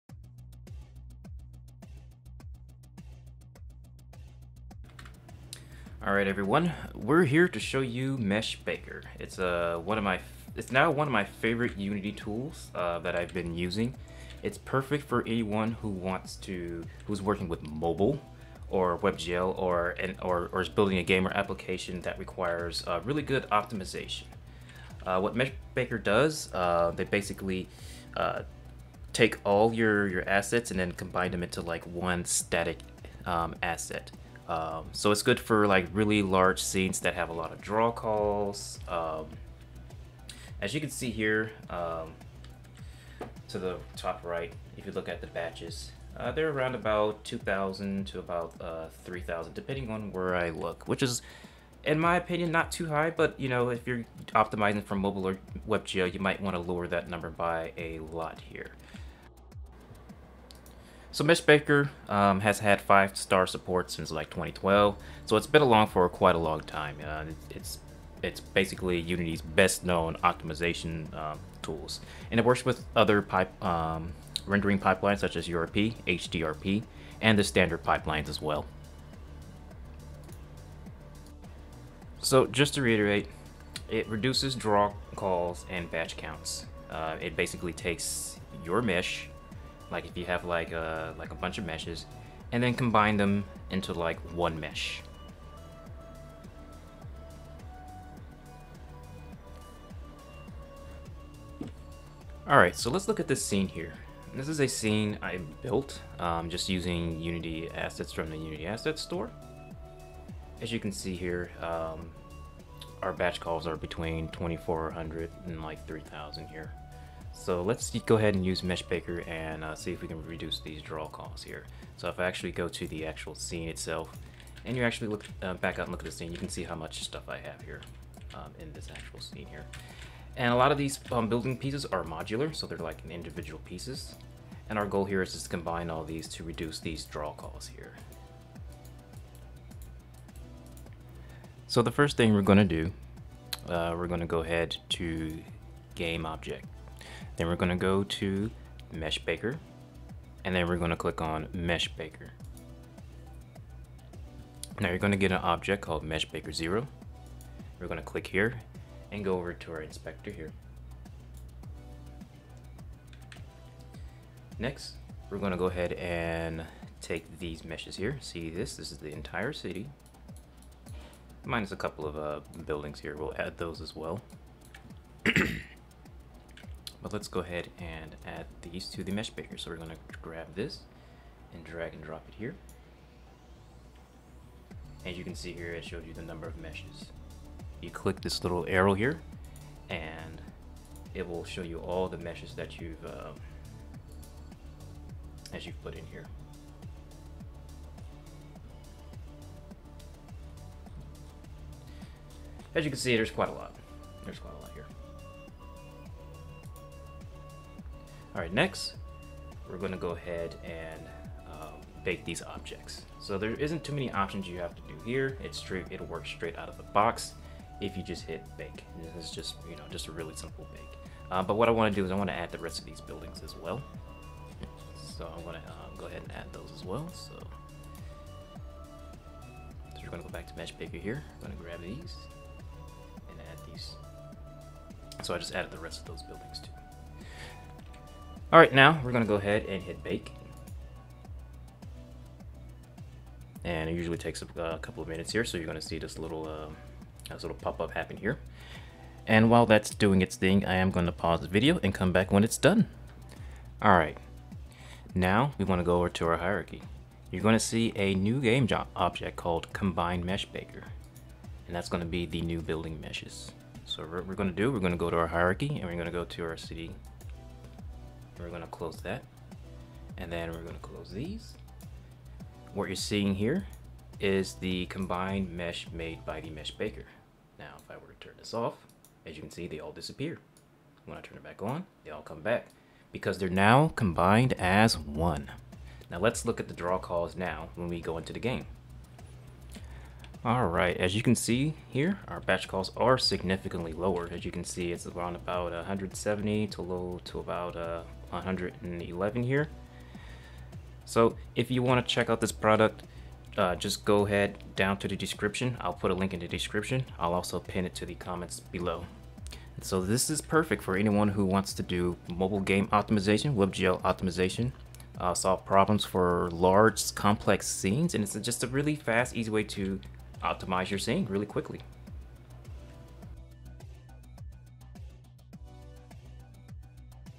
Unity 3D Game Design | Optimize Large Scenes
Mesh baker allows you to quickly optimize large Unity 3D scenes for your game design project. Mesh baker combines multiple meshes to make them into one, reducing batch calls.

In this video we look at Mesh Baker for Unity 3D. I do a quick tutorial on how to quickly optimize a large complex scene for mobile webgl and more. This is perfect for anyone who is developing a game for mobile device or platforms that require optimization.